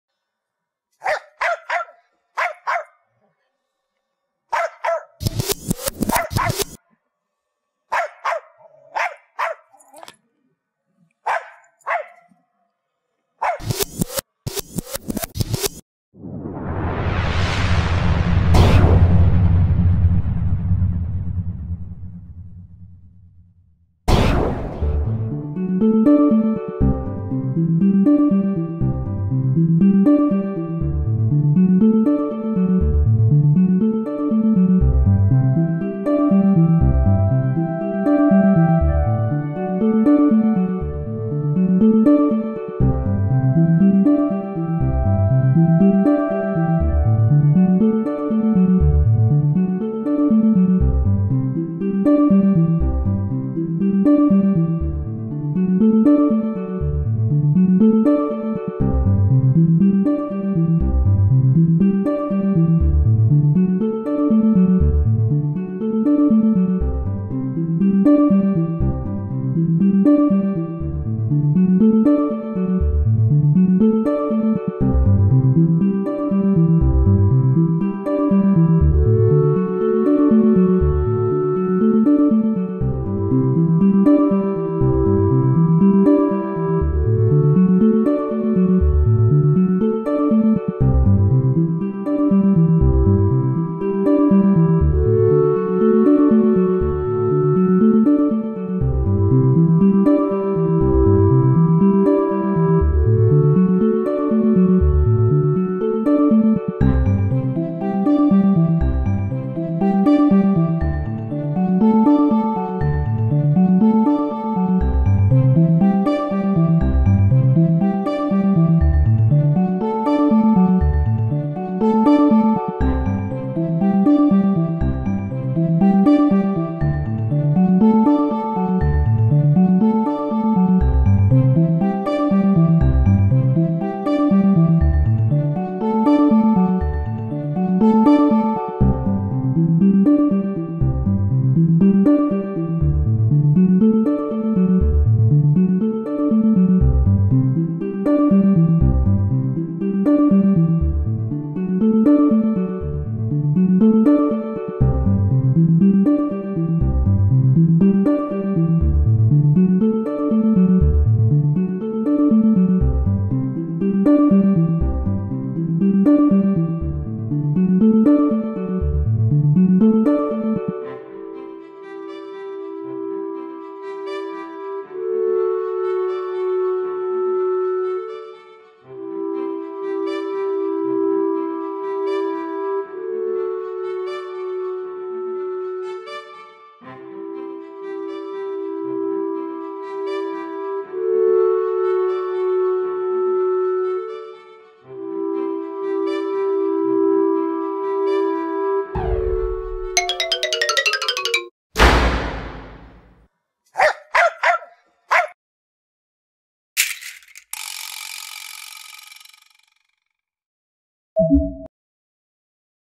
yogi - a schipperke's life
A tribute to my Schipperke, Yogi. The video features his life at home and on the boat. If possible, watch in High definition for the quality.